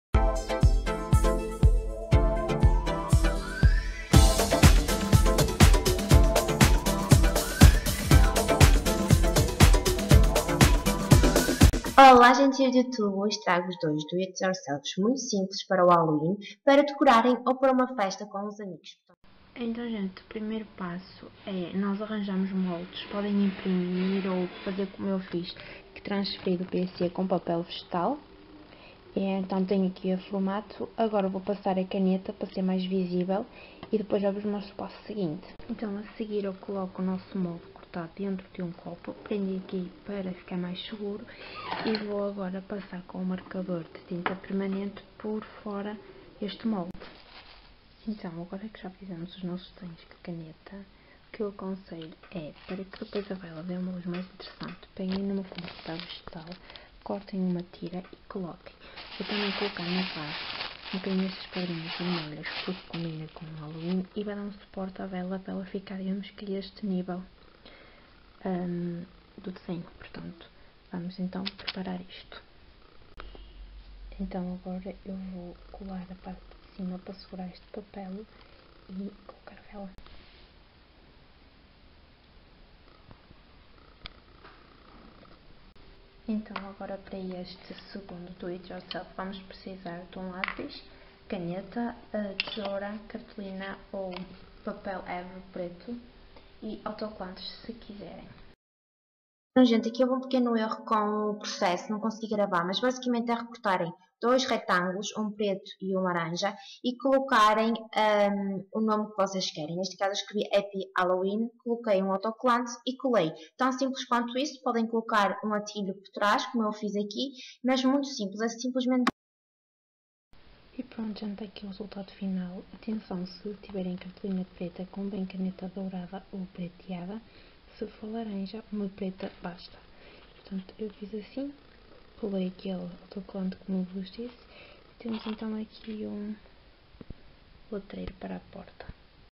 2 diy para halloween
olá gente gira, o vídeo de hoje será dedicado ao halloween e como tal preparei dois DIY para decorarem o vosso quarto/casa ou para uma mini festa entre amigos. São dois DIY muito simples que toda a gente consegue fazer